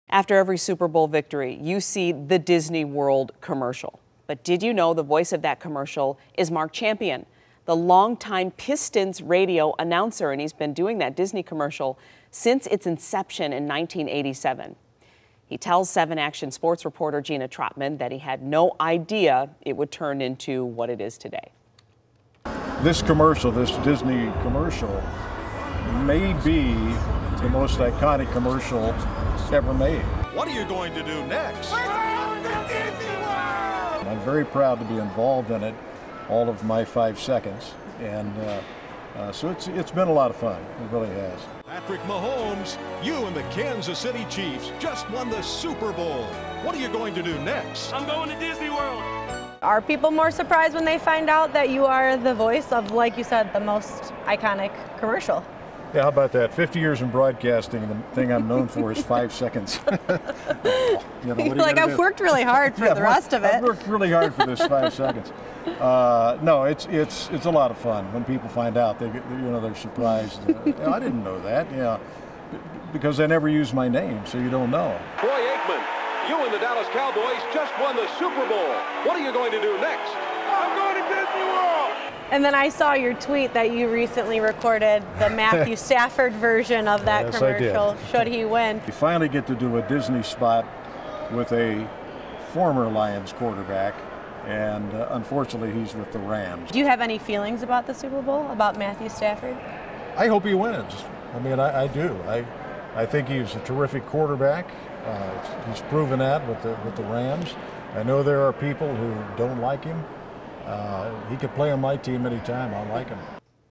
Iconic 'I'm going to Disney World' commercial voiced by Pistons broadcaster Mark Champion
You know the iconic "I'm going to Disney World" Super Bowl commercial, but did you know it's been voiced every year by Pistons broadcaster Mark Champion? Jeanna Trotman reports.